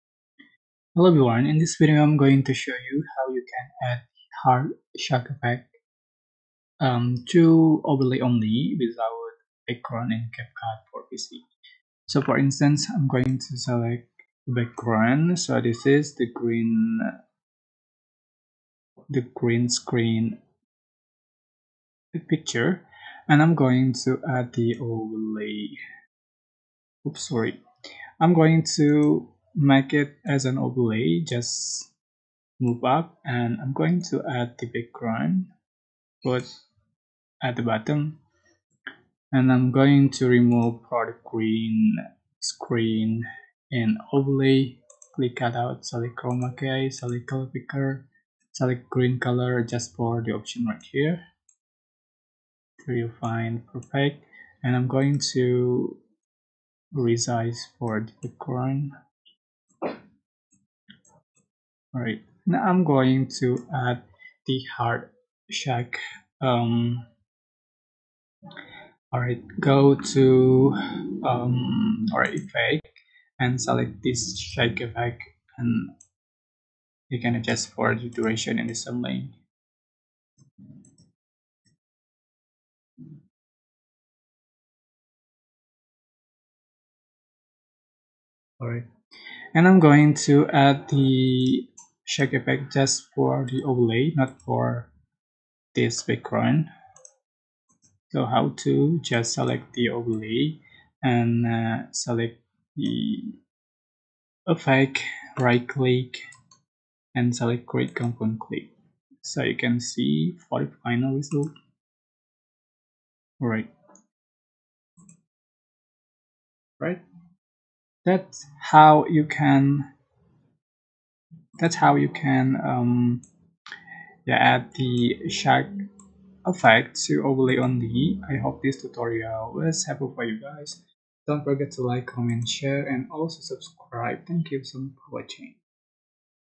How to Apply Hard Shake Video Effect to Overlays Only in CapCut PC? CapCut Tutorial on PC!
Want to Apply Hard Shake Video Effect to Overlays Only in CapCut PC? Here's how you can do that